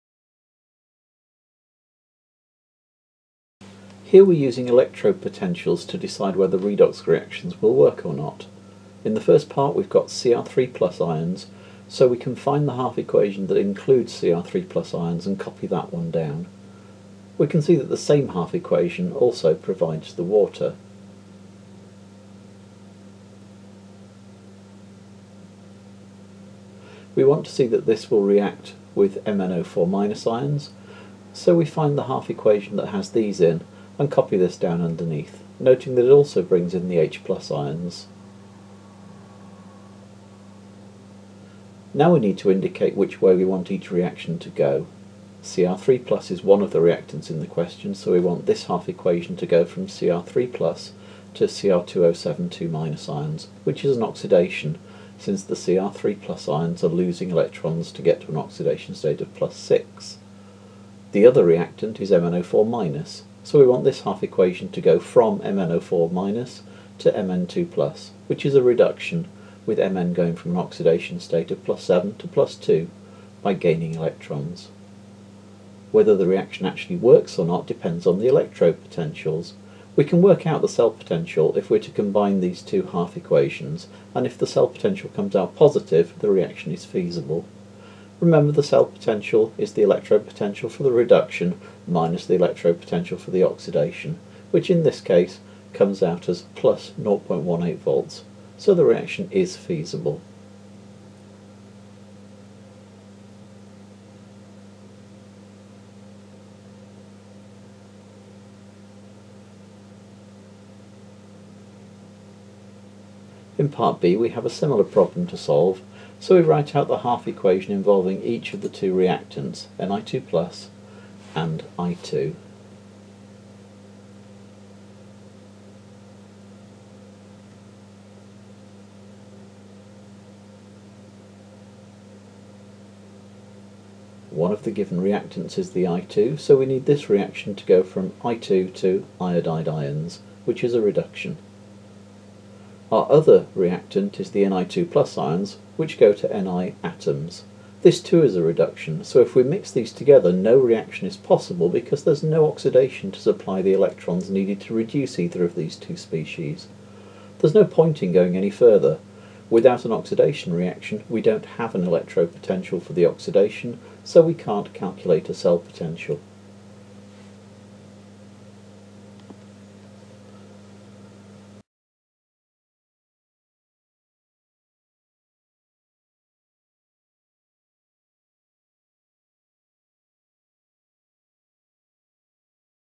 How2: Use electrode potentials to determine the feasibility of redox reactions
Working out the cell potential for a proposed redox reaction makes it possible to suggest whether that reaction would be feasible.